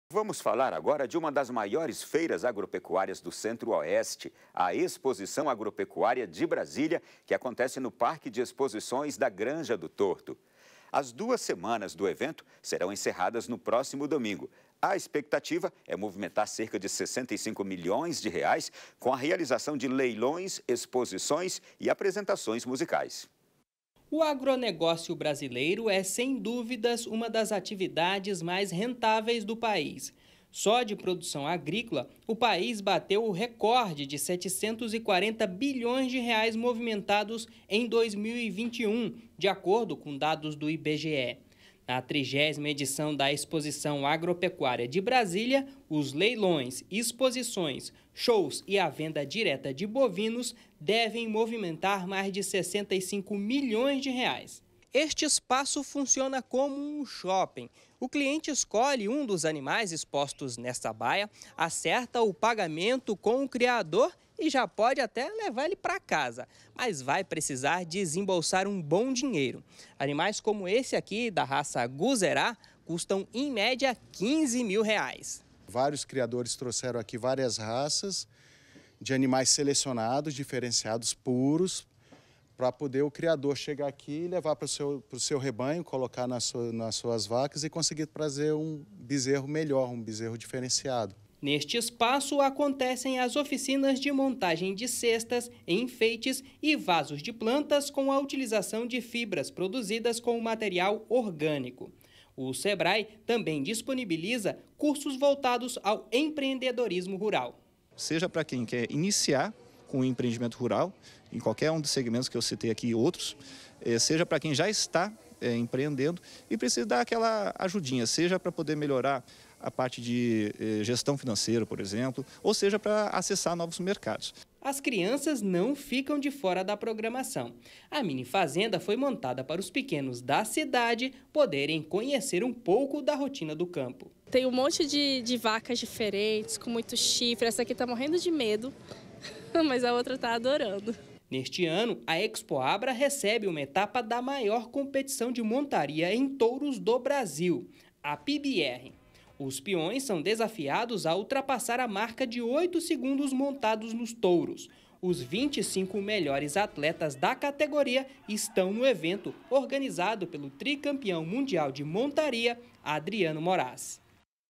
Exposição Agropecuária de Brasília termina no fim de semana
O assunto dessa reportagem é uma das maiores feiras agropecuárias do centro-oeste: a Exposição Agropecuária de Brasília, que acontece no Parque de Exposições da Granja do Torto. As duas semanas do evento serão encerradas no próximo domingo. A expectativa é movimentar cerca 65 milhões de reais com a realização de leilões, exposições e apresentações musicais.

Reportagem de Francisco Coelho
Imagens de Ersomar Ribeiro